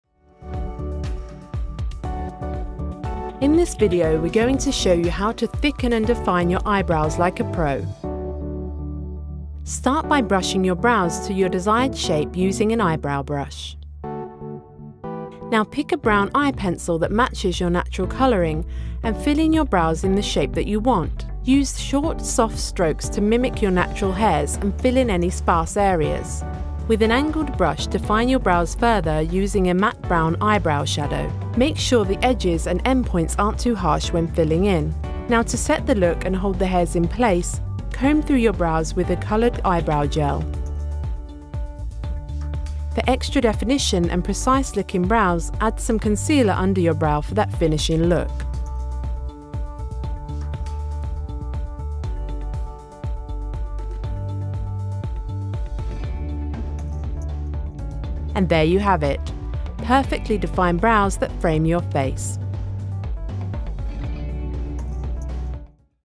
Jak zesílit obočí? | Oriflame kosmetické tipy
Hustá a výrazná obočí jsou v módě. Podívejte se, jak vytvořit dokonale vyklenutá obočí – ani ne za minutu!